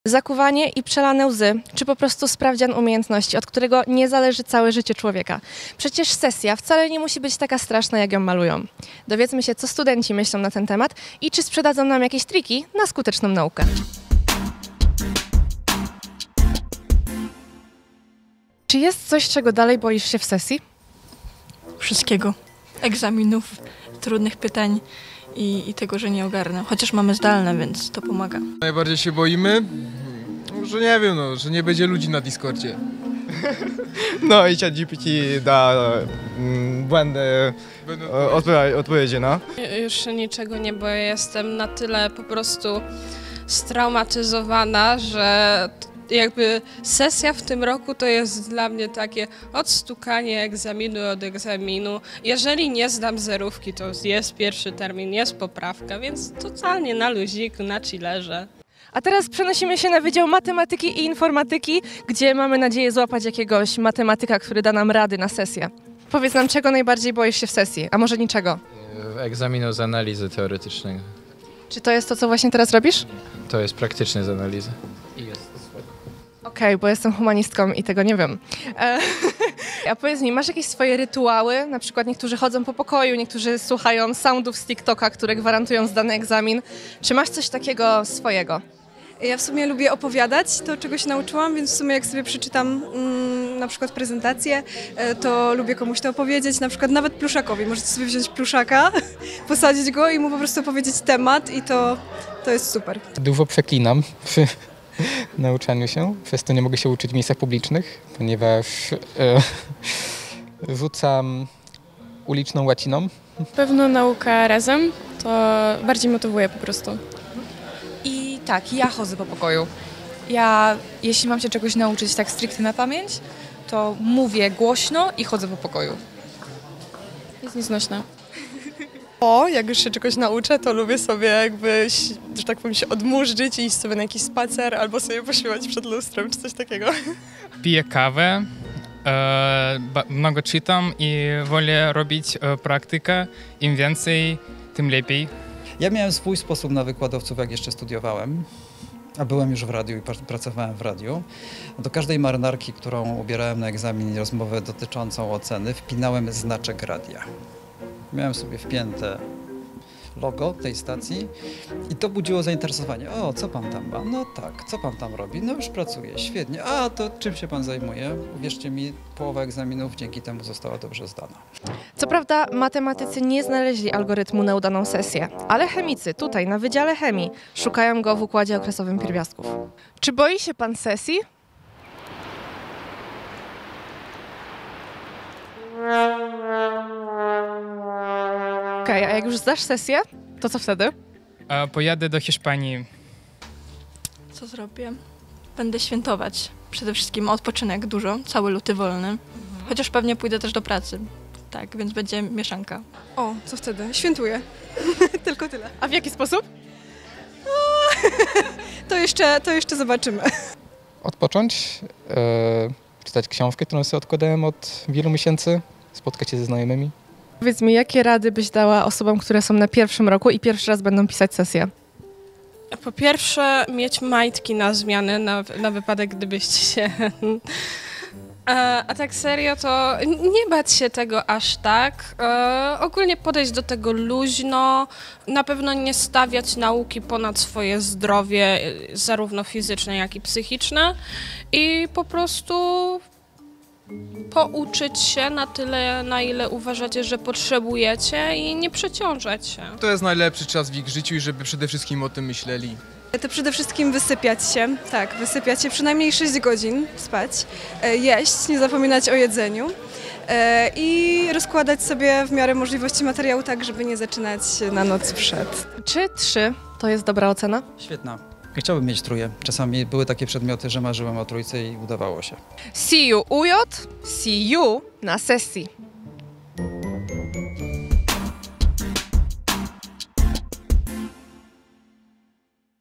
Czy sesja jest taka straszna jak ją malują?
Sesja zimowa trwa. Można by powiedzieć "winter is coming"❄️. Ale w tym materiale znajdziecie wiele wskazówek na to, jak poradzić sobie z tym trudnym i stresującym czasem!😎Sesja wcale nie musi spędzać wam snu z powiek! 😴Życzymy powodzenia i połamania piór!🍀